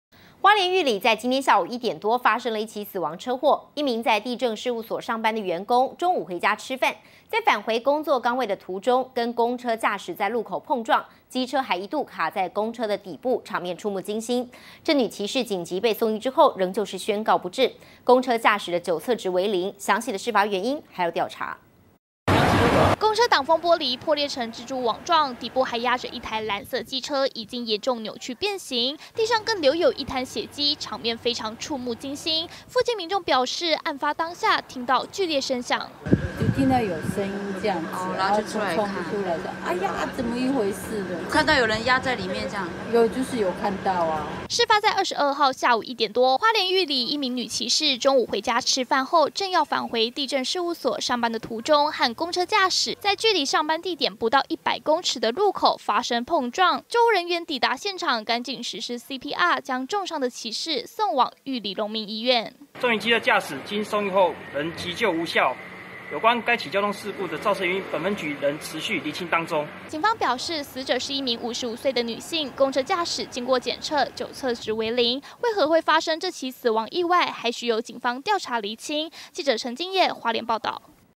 地政所員工擦撞公車慘死 離上班地僅百公尺
花蓮玉里今天(22號)下午一點多，發生死亡車禍！一名在地政事務所上班的員工，中午回家吃飯，在返回工作崗位途中，和公車駕駛在路口碰撞，機車還一度卡在公車底部，場面怵目驚心！女騎士緊急送醫後，仍然宣告不治。公車駕駛酒測值為0，詳細事發原因，還要警方調查釐清。